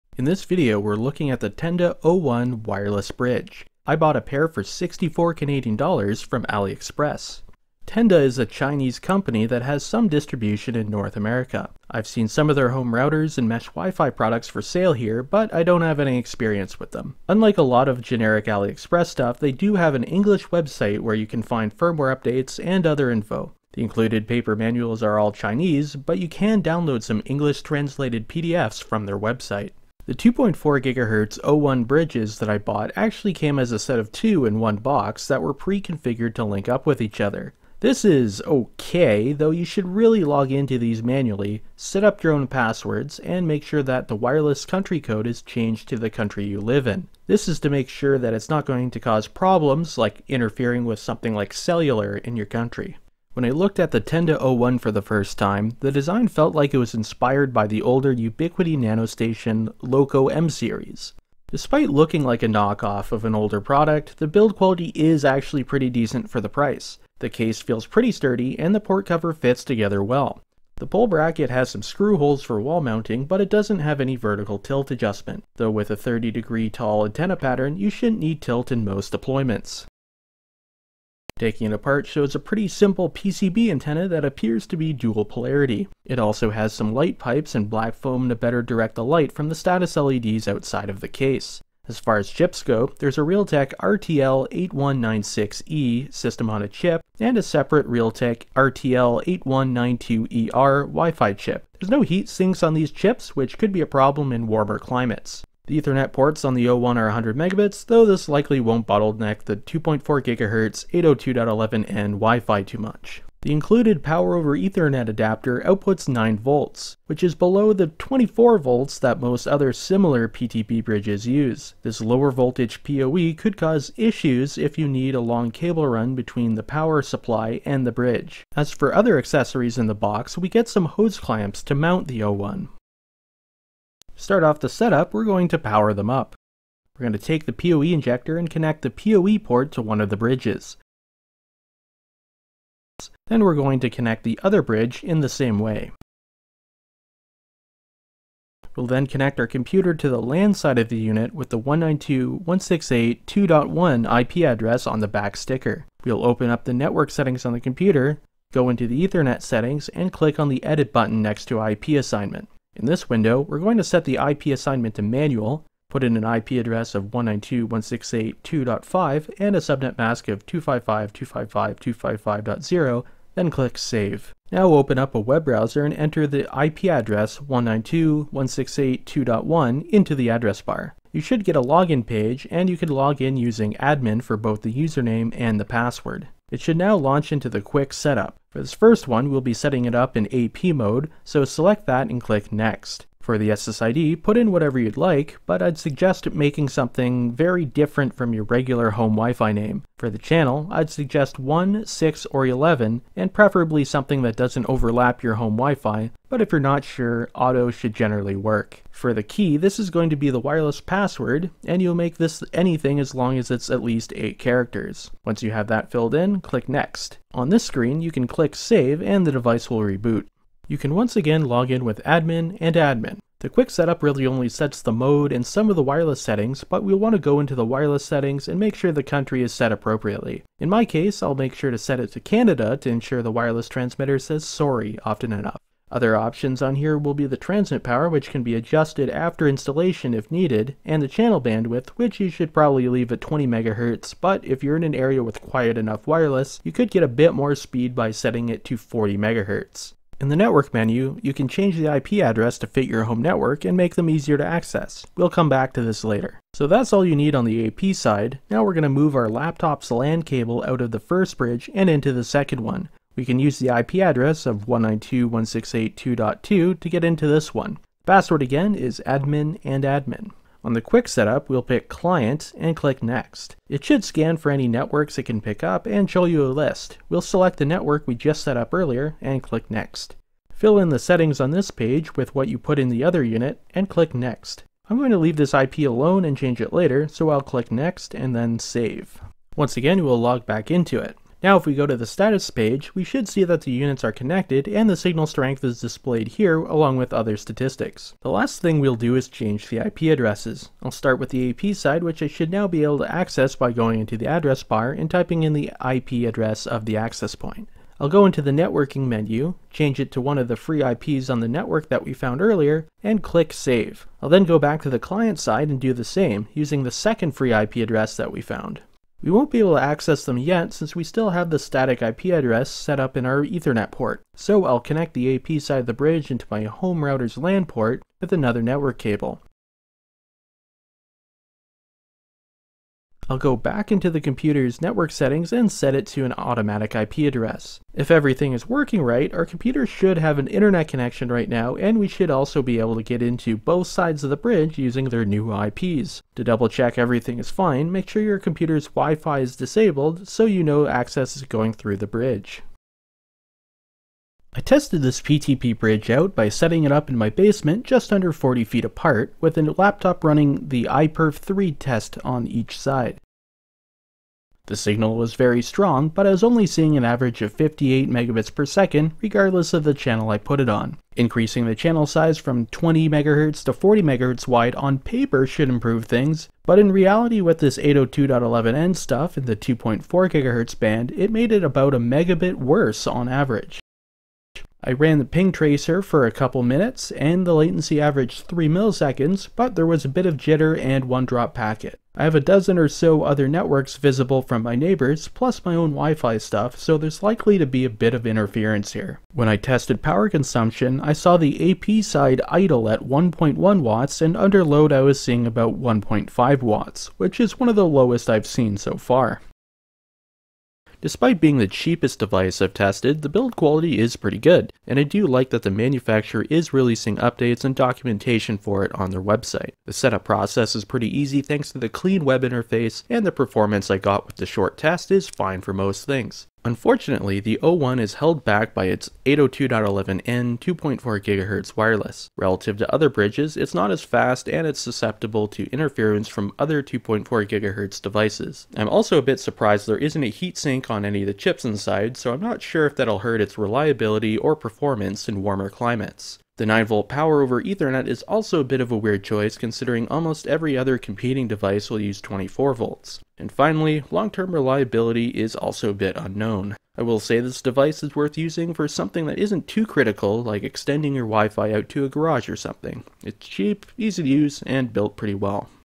Bridging on a Budget: Tenda O1 - The Cheapest 2.4GHz WiFi Bridge on Aliexpress
The Tenda O1 was the cheapest WiFi bridge that I could find on Aliexpress. The build quality is surprisingly good, but where did they cut the corners? I will go through the setup, take it apart, and do some benchmarking in this video.

The price can get even cheaper than what I got, try searching for "Tenda O1", this link is only an example: